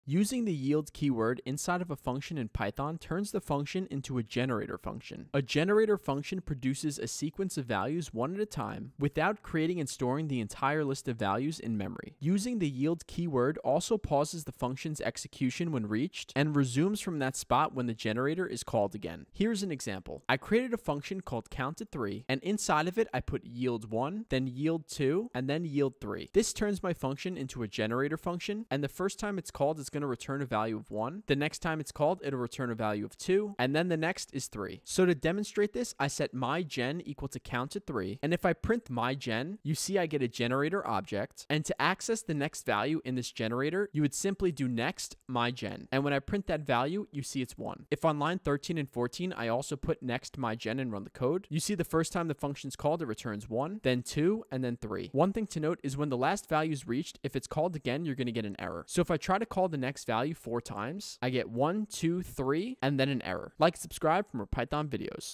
How Yield Turns Functions Into Generators | Python Tutorial
The yield keyword in Python transforms a function into a generator function, which produces values one at a time without storing the entire list in memory. When the function reaches yield, it pauses and returns a value, resuming where it left off when called again. This makes generators ideal for working with large data or infinite sequences efficiently.